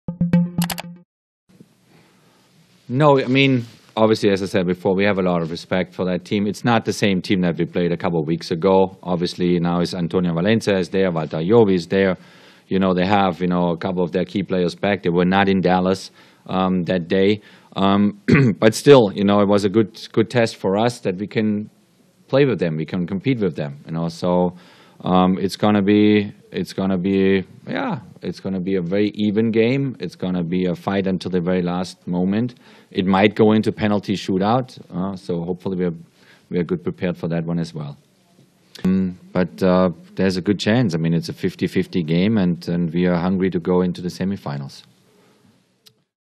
Klinsman asegura que "están hambrientos por llegar a semifinales"
Seattle (EE.UU.), 15 jun (EFE).- El seleccionador de Estados Unidos, el alemán Jürgen Klinsmann, pronosticó que los cuartos de final ante Ecuador "se lucharán hasta el último suspiro", mostró "mucho respeto" por la selección sudamericana y confesó que están "hambrientos" por pasar la eliminatoria.